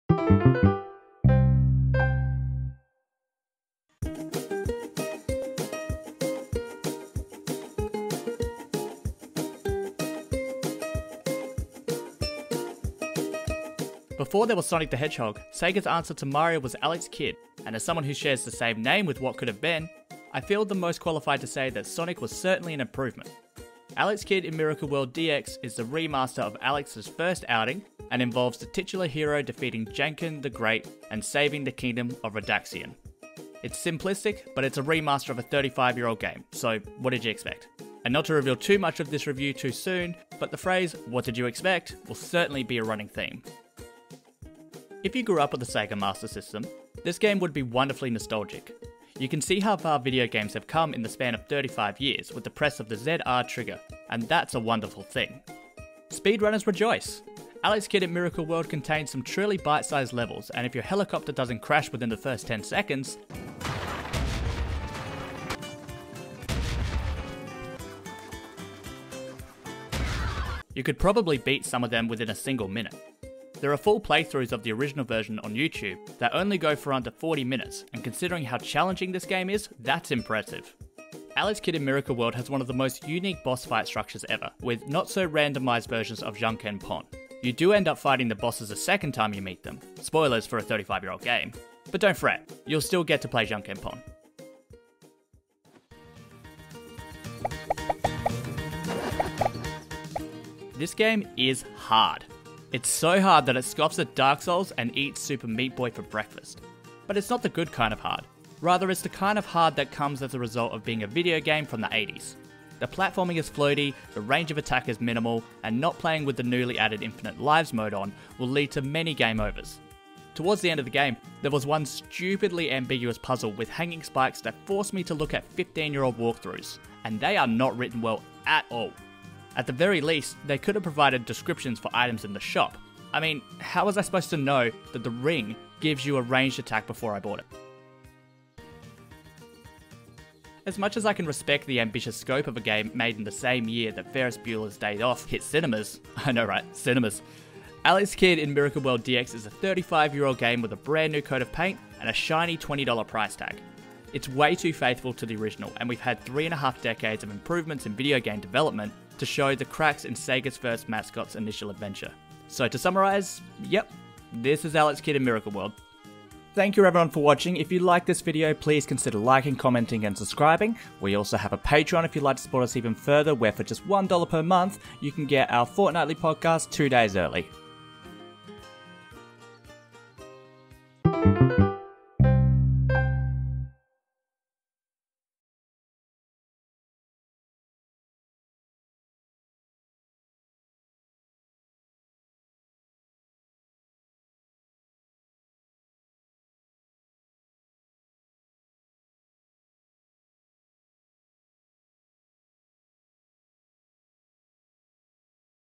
Alex Kidd in Miracle World DX - Switch Review
With the game being a cut and paste of the original with a new coat of paint plastered over the top, this review of Alex Kidd in Miracle World DX maintains the theme "what did you expect?" throughout.